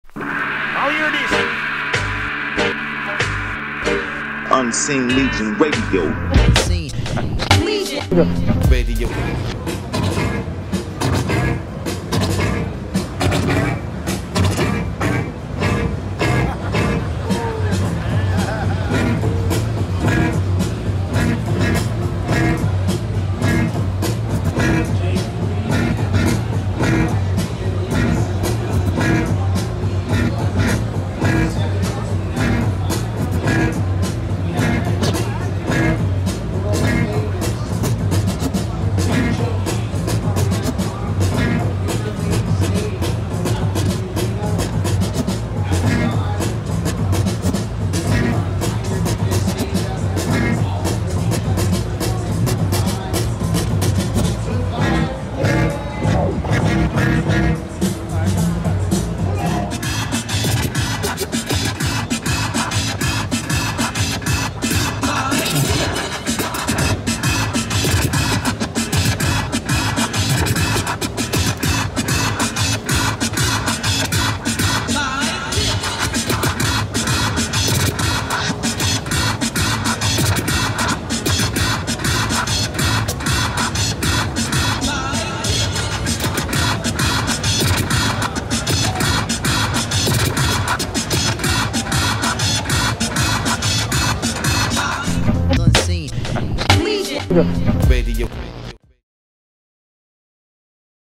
DJ Rob Swift and DJ Mista Sinista beat juggling back to back
Two of the legendary X-Ecutioners taking to the turntables at the Reloop booth at the NAMM 2020